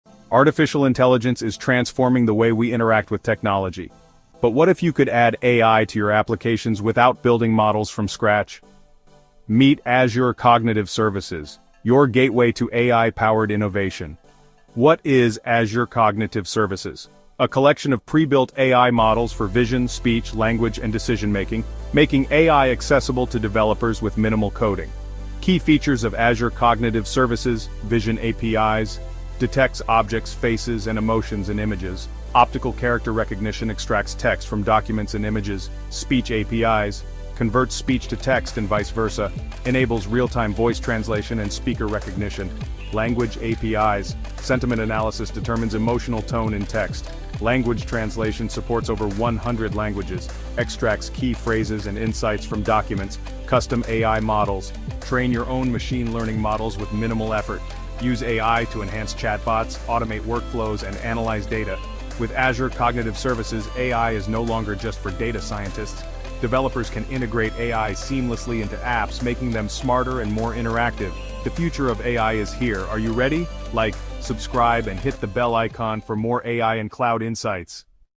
Azure Cognitive Services – AI-Powered Innovation for Developers
🚀 Bring AI to Your Applications with Azure Cognitive Services! 🤖

Azure Cognitive Services provide ready-to-use AI models for image recognition, speech processing, language understanding, and decision-making—all with minimal coding. Whether you're building smart chatbots, analyzing images, or automating workflows, Azure makes AI accessible to everyone!

🔥 In This Video, You'll Learn:
✅ How Azure Vision APIs analyze images and detect objects
✅ How Speech APIs enable real-time speech-to-text and voice recognition
✅ How Language APIs translate text, analyze sentiment, and extract insights
✅ How to create custom AI models effortlessly

📌 Azure Services Covered:
👁️ Azure Vision API – Image recognition & object detection
🗣️ Azure Speech API – Voice recognition & text-to-speech
📖 Azure Language API – Sentiment analysis & translation
🧠 Custom AI Models – Train AI models with minimal effort